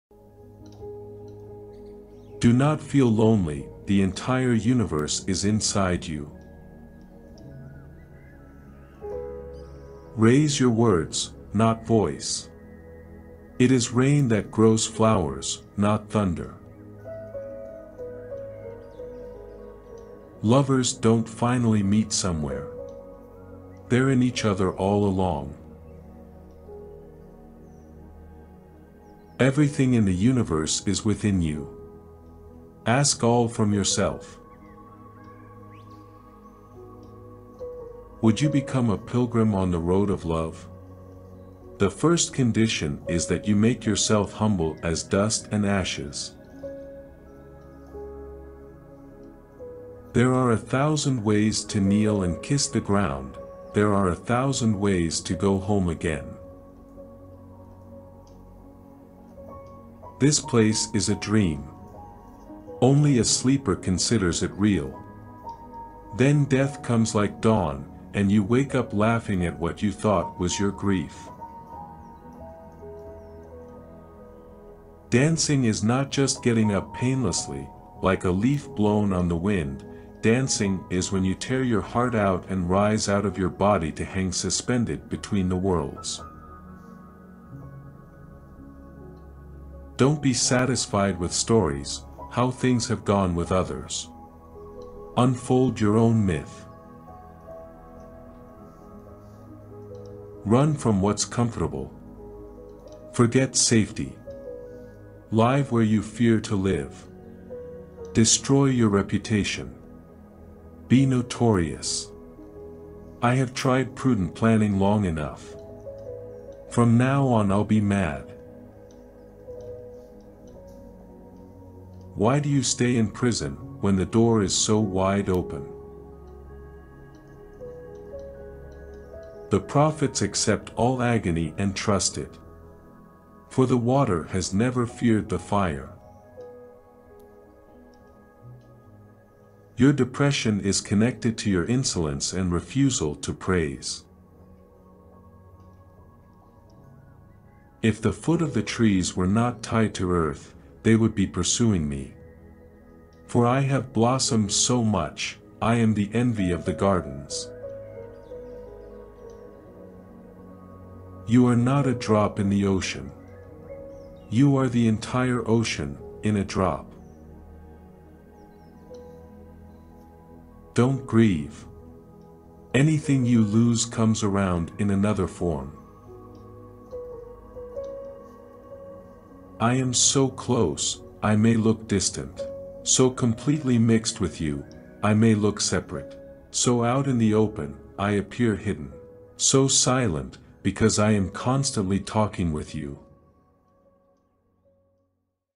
Rumi Quotes on Life, Love, and Time that are Worth Listening To ! Inspiration Quotes
If you need some help seeing the bigger picture of love and life, these Rumi quotes and sayings will expand your mind in ways you never knew. Gain insight and inspiration from a great wordsmith that lived in the 13th century.
Rumi was a 13th-century Persian poet who had a massive influence on Muslim writing and culture. He was born on 30 September 1207, either in Wakhsh, a village in present-day Tajikistan, or in the city of Balkh, in present-day Afghanistan.
Rumi has a huge following around the world and is actually one of the best-selling poets in the USA. His works have been translated into many different languages, including Spanish, Turkish, German, Russian, Urdu, Arabic, French and Italian.

Keep these amazing quotes bookmarked on your phone or computer for quick access anytime you need a pick-me-up.